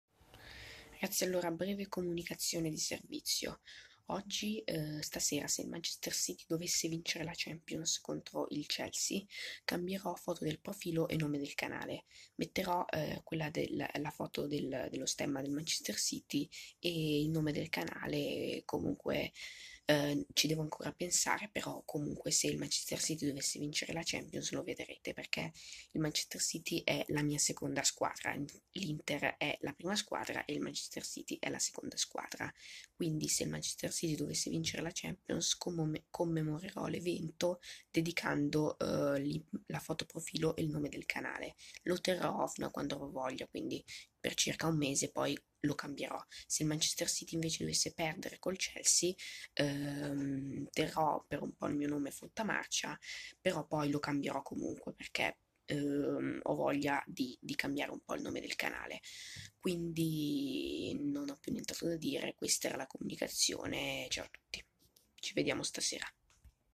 Comunicazione di servizio: se il Manchester City dovesse vincere la Champions...
In realtà sta sera non ci vedremo, ma bensì domani con la foto profilo é il nome del canale (spero) aggiornati